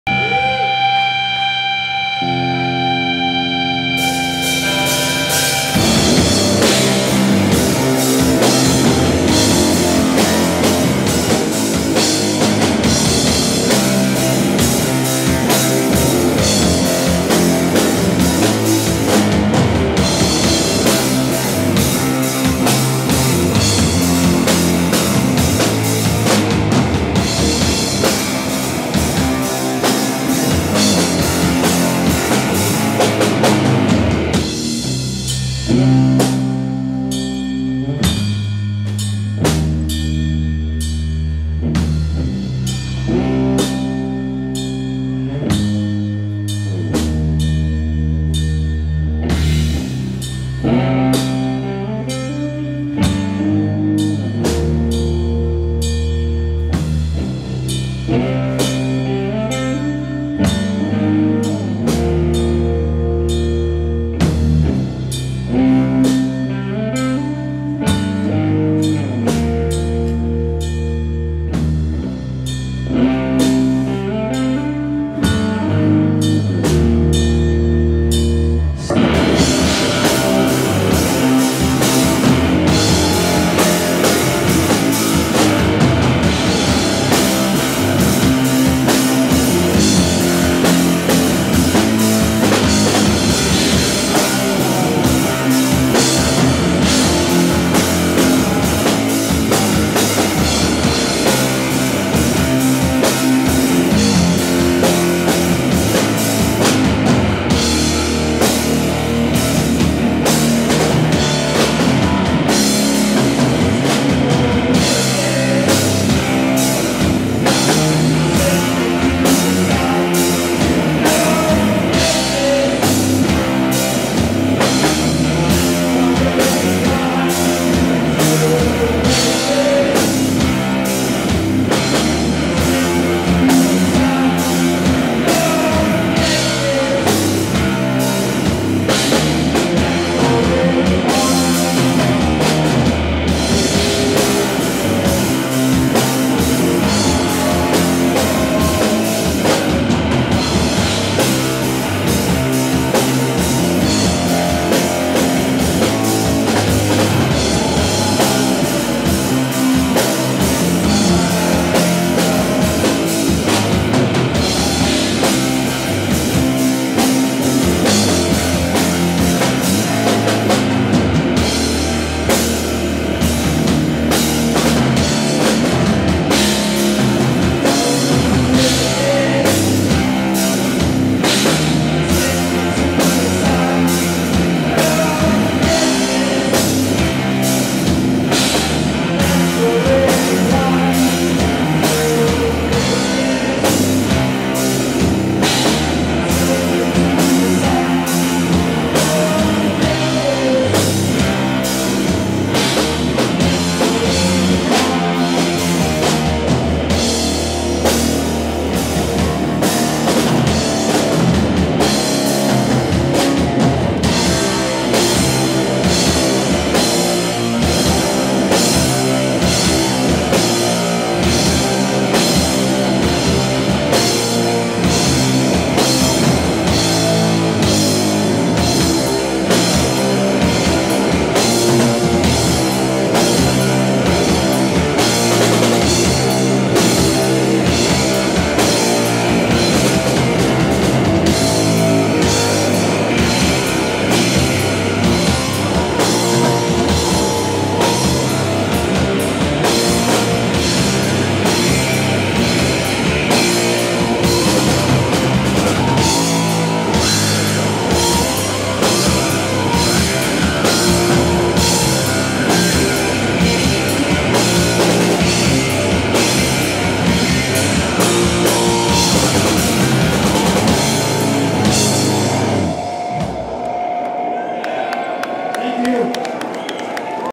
FOSTERMOTHER play Ripplefest Texas
Filmed by Billy Goate, Editor in Chief of Doomed & Stoned on Saturday afternoon, August 7th, 2021 at Texas Ski Ranch in New Braunfels, Texas.